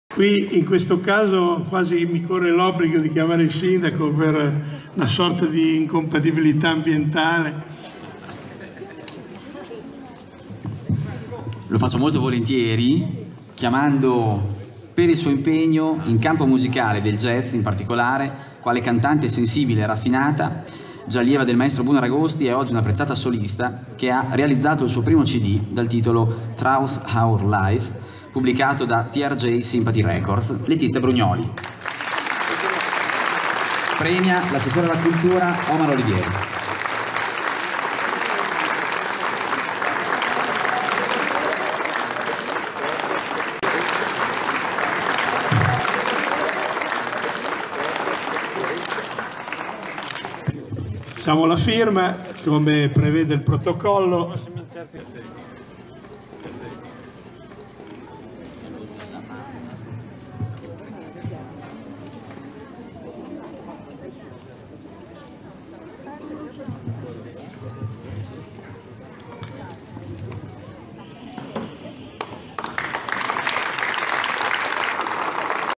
Madonna del Carmine 2014 Cerimonia Benemerenze Letizia Brugnoli - “Through our life” Borgotaro
"Madonna del Carmine 2014: Cerimonia di consegna Benemerenza a Letizia Brugnoli (“Through our life”) - Borgotaro (Parma) - 19 luglio 2014"